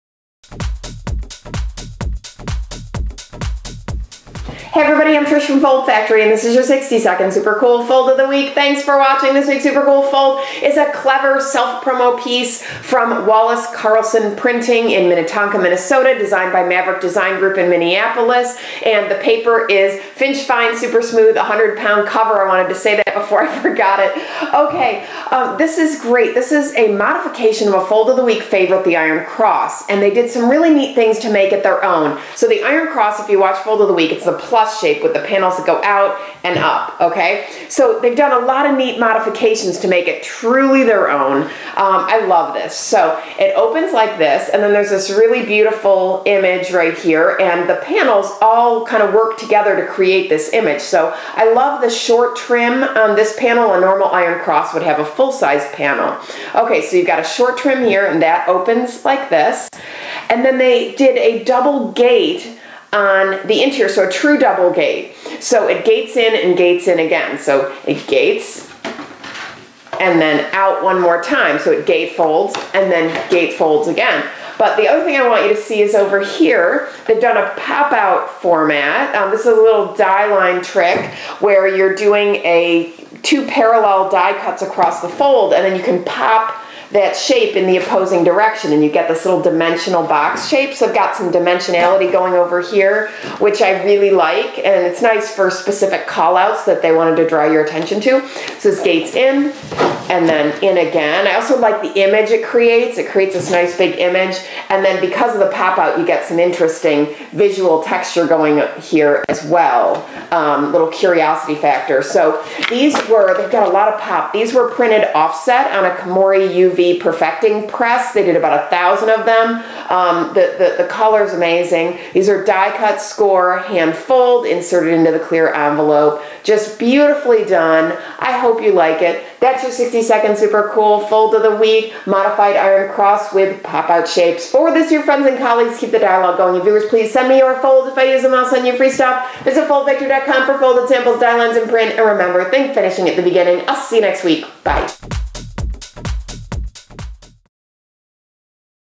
FOW #364: “Make It Your Own” Folded Cross
“Make It Your Own” Folded Cross. Trish Witkowski shares her super-cool folding samples and helpful production tips. for folding ideas, downloads, and templates.